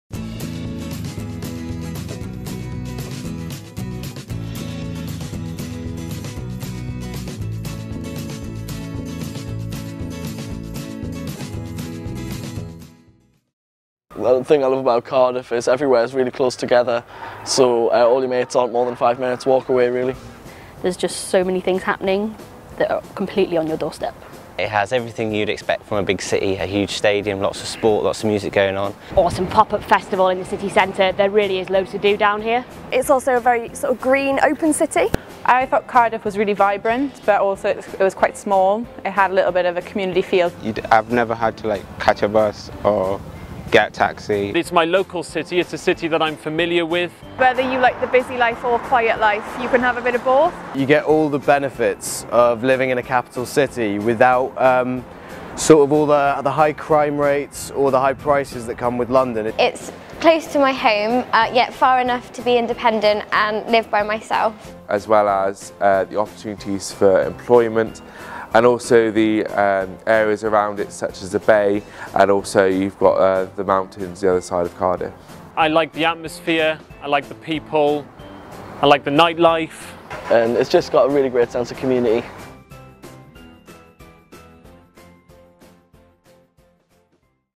The City of Cardiff
We asked some current students what they think is the best thing about Cardiff as a city.